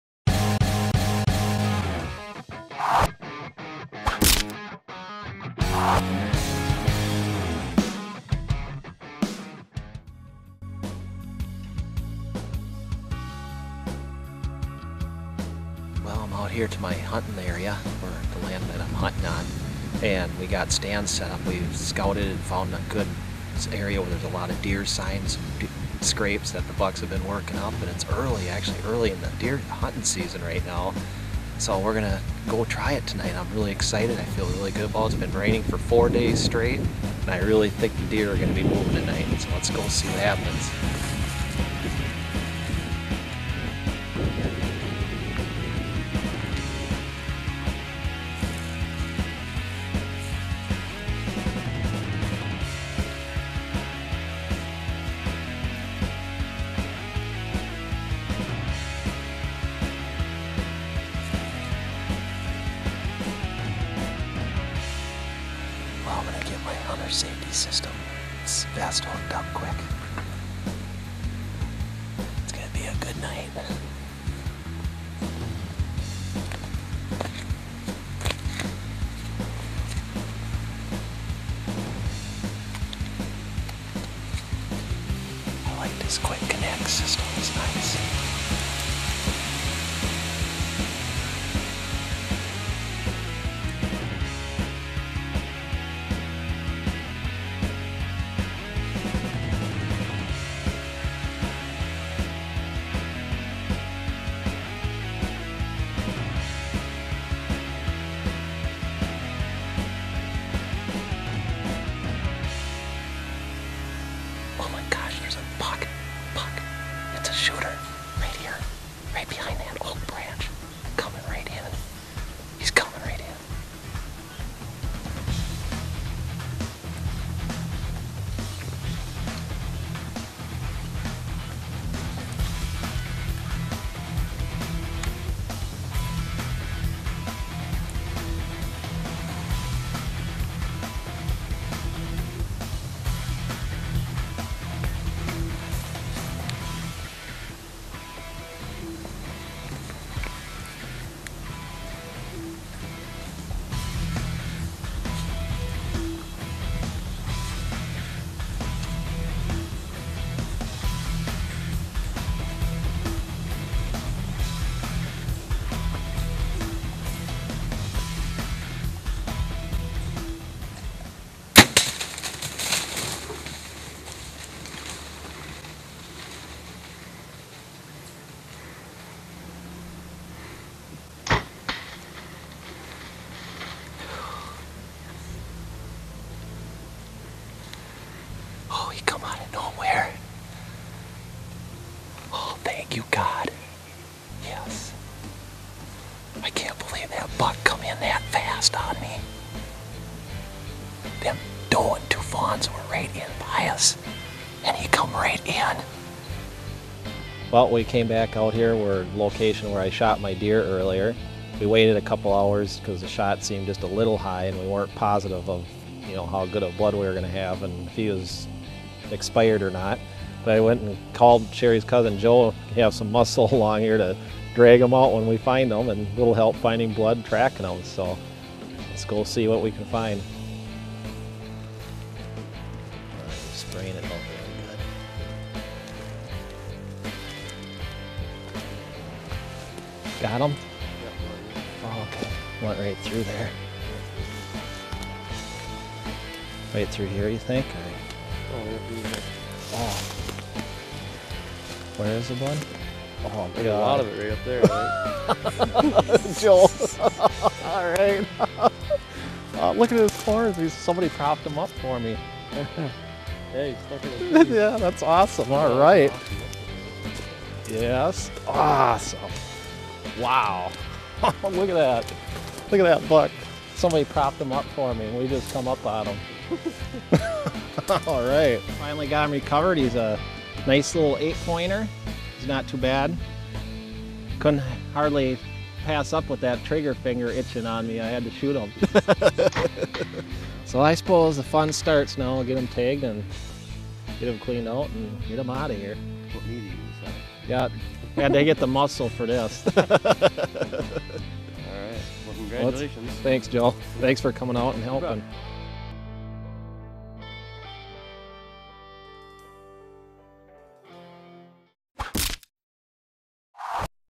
WI 8 pt buck arrowed from tree stand bow hunting the rut during archery season Calling bucks
Shari & Scott Scribner of the Nature Zone submitted this video taken in their home state of Wisconsin while hunting White tailed deer DIY. In this video Scott takes an 8 point buck with his wife Shari behind the camera capturing the action including an awesome arrow impact! Congratulations from Paul Korn and the gang at A-1 Archery & Tombstone Creek Outfitting